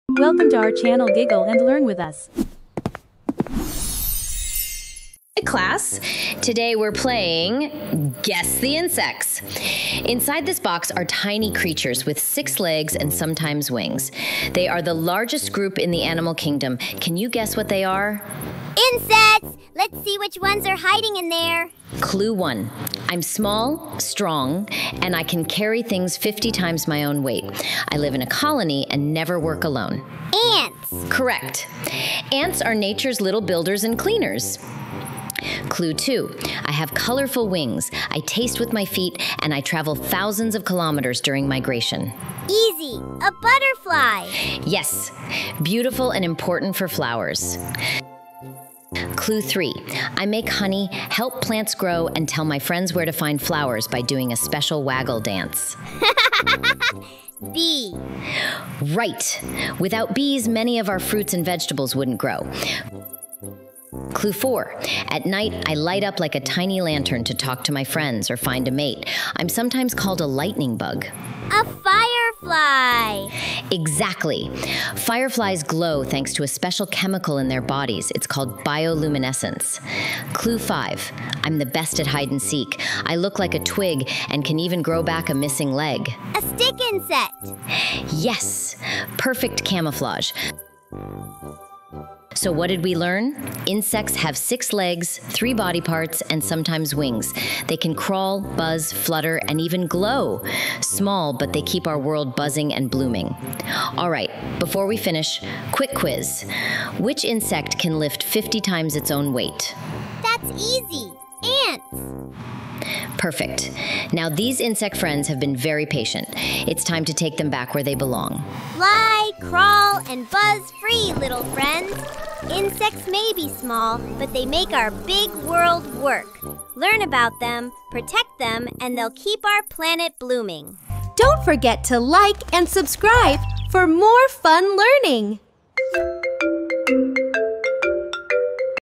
Guess the Insects for Kids 🐜 Fun facts about Insect with Anshu
Join Anshu in a fun classroom guessing game to learn all about amazing insects! 🐜🦋🐝 From ants and butterflies to bees, ladybugs, stick insects, and glowing fireflies — discover cool facts, what makes them insects, and why they are so important for our planet.

🌟 Key Learning Points:

What makes an insect an insect (six legs, three body parts, sometimes wings)

Fun facts about 6 popular insects kids love

Why insects are the largest group in the animal kingdom

How insects help nature stay healthy

Perfect for kids, parents, and teachers looking for fun and educational videos about animals and nature!